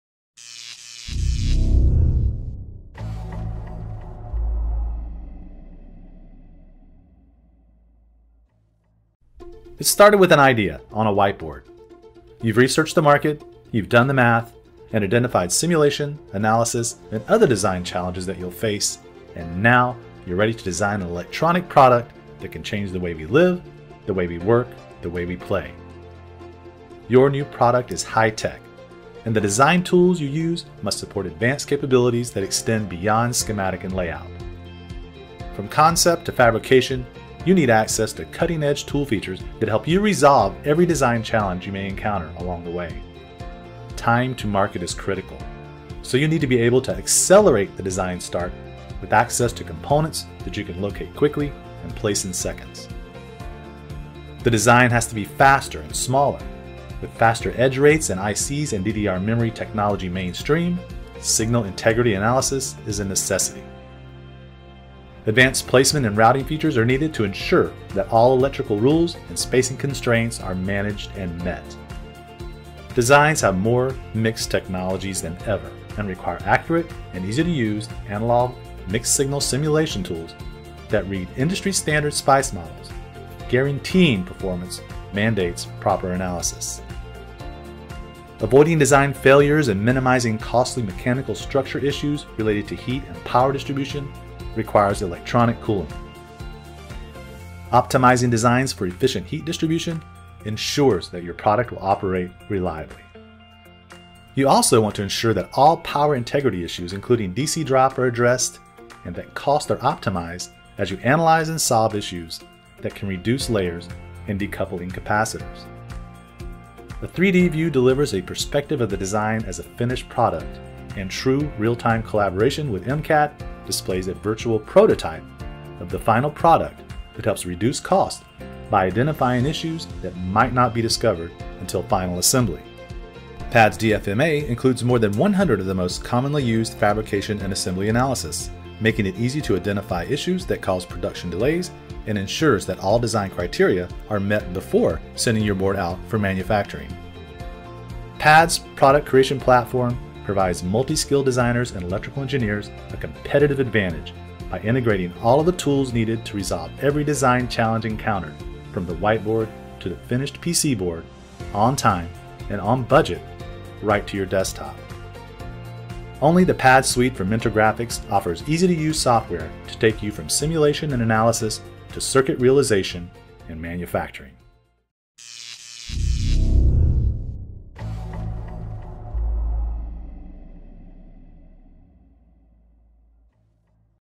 PADS® Product Creation Platform
From concept to fabrication, electronic product creation is more than schematic and PCB layout. Engineers and small teams must solve design challenges ranging from component selection, signal and power integrity, electronics cooling, form, fit, and function, to layout and manufacturing. Get from simulation and analysis to circuit realization and manufacturing quickly and easily...with PADS. for more information.